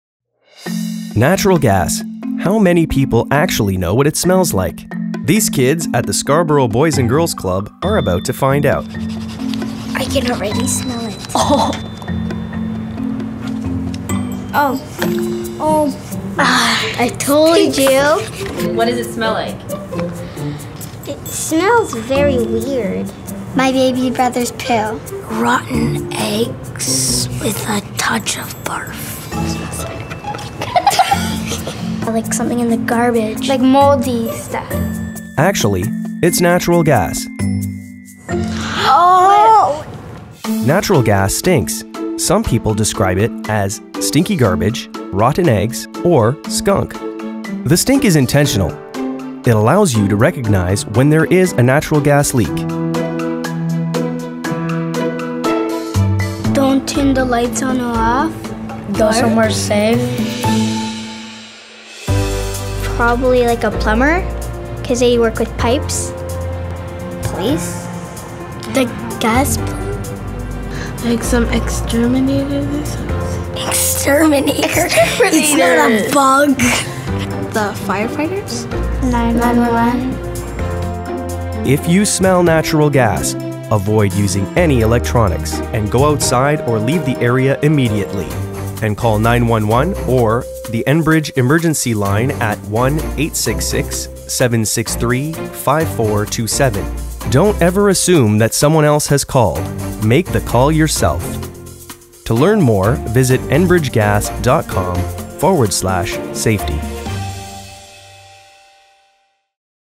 What does natural gas smell like?
This past summer, Enbridge Gas visited a Boys and Girls Club in Scarborough to give them a whiff of what natural gas smells like.

The best way to describe the smell is rotten eggs, but it can also be identified as a blend of other things, such as: Sulphur, garbage, and boiled eggs. However it’s described, the smell is distinctive and intentional so that people notice and can act appropriately.  to request a natural gas scratch 'n sniff.